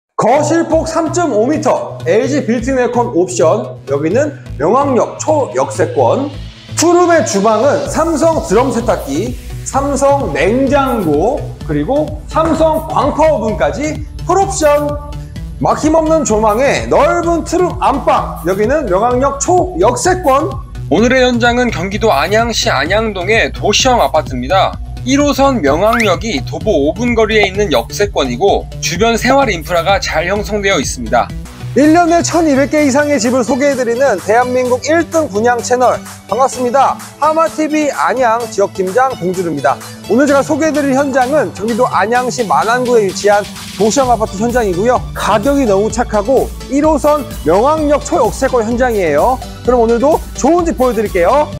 경기 안양시1호선 명학역 초역세권 2억대 2룸 풀옵션~! 매물번호4619#Shorts
안녕하세요^^

구독자 여러분의 안전한 주택분양 길잡이
하마tv의 봉쥬르 입니다..^^

저희 하마티비의 꿀매물들을
가장 빨리 보고 싶은 분들은
눌러 주시기 바랍니다

◐위치◐

경기도 안양시 안양동

◐타입◐

2룸 기준층 실13평~18평

◐특징◐

1개동 29세대 도시형아파트

안양 최고의 인프라

명학역(1호선) 도보5분

세탁기,냉장고, 광파오븐, 시스템에어컨 풀옵션

자주식주차장, CCTV, 엘리베이터

층간소음방지재, 내진설계, 친환경자재

- 봉주르의 말 -

명학역(1호선) 초역세권 도보5분

안양 투룸 착한가격

확 트인 조망권

★분양가★

2룸 기준층

2억후반~

★실입주금★